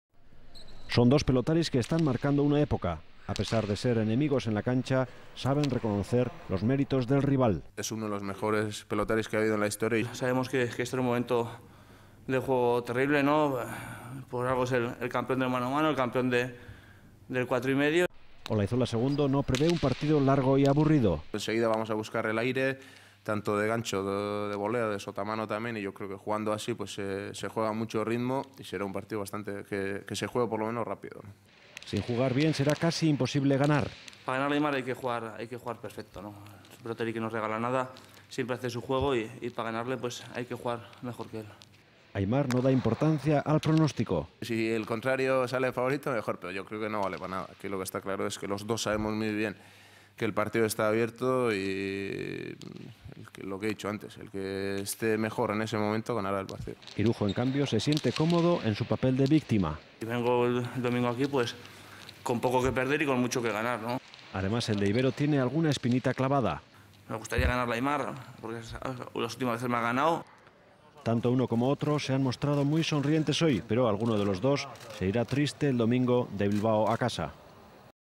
Olaizola II- Irujo, comienza la cuenta atrás
El enfrentamiento entre Aimar Olaizola y Juan Martínez de Irujo ha despertado una gran expectación entre los aficionados.